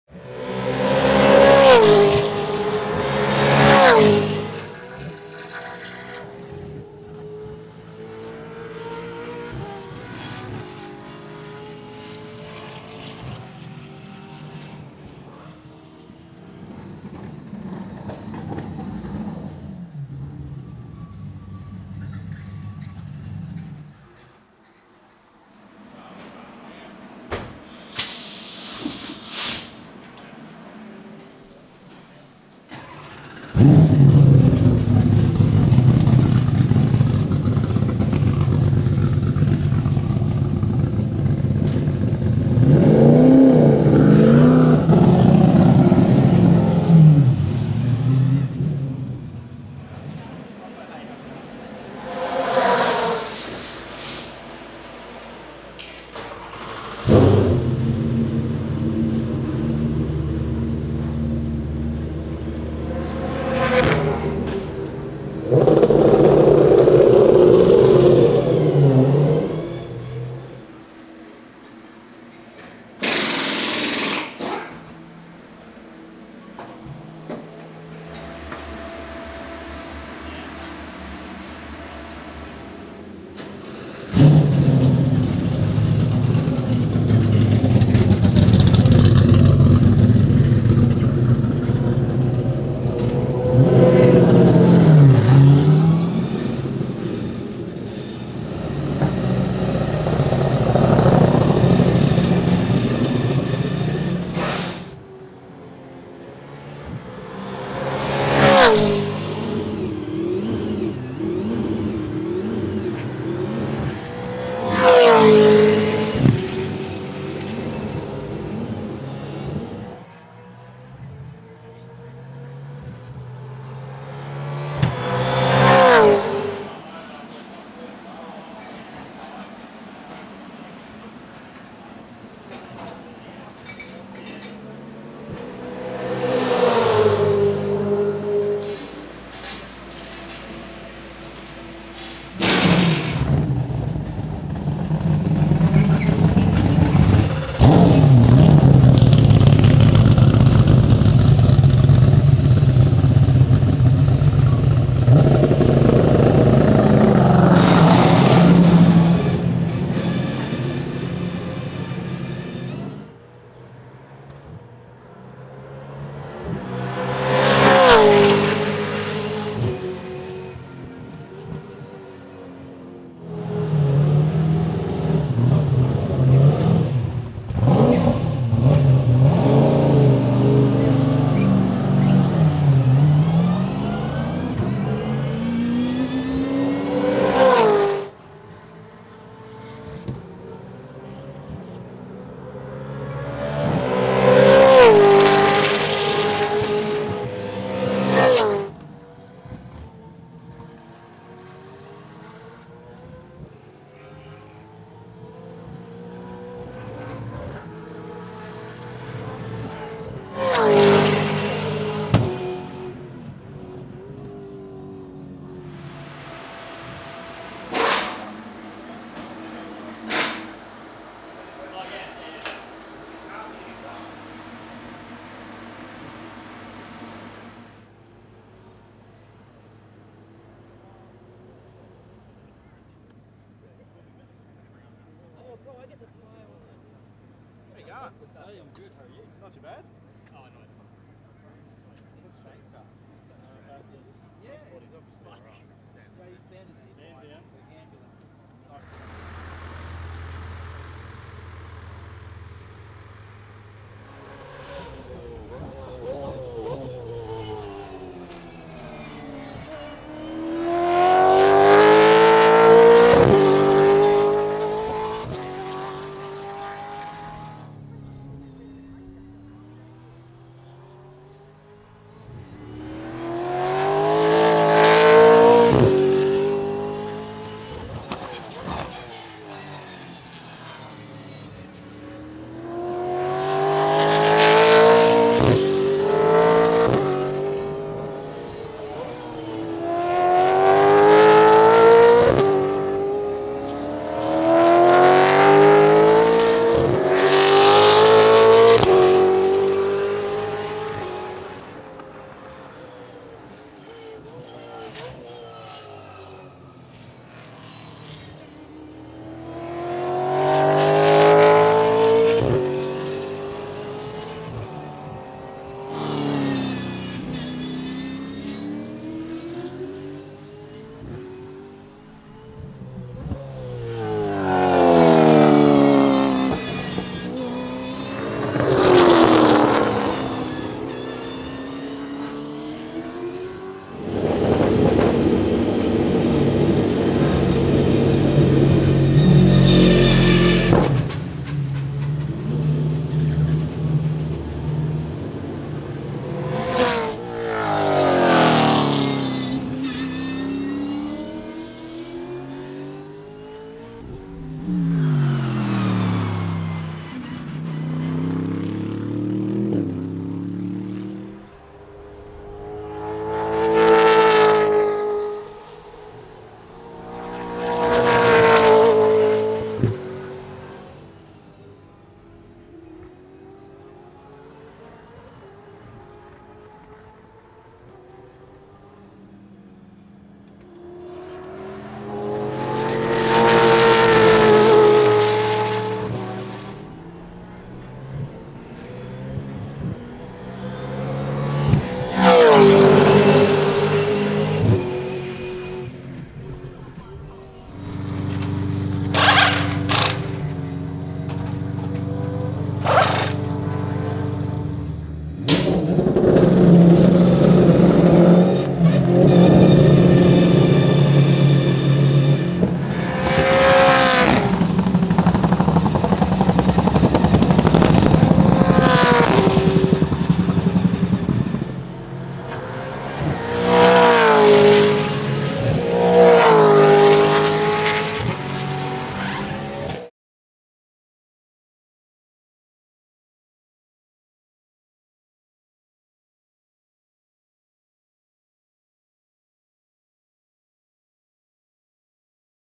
Sights & Sounds: Supercars enduro test at Queensland Raceway
Queensland based PremiAir Racing, Shell V-Power Racing Team, Matt Stone Racing & Red Bull Ampol Racing ahead of the Sandown 500.

The session was red-flagged after Shane van Gisbergen suffered a spectacular brake failure at turn 6.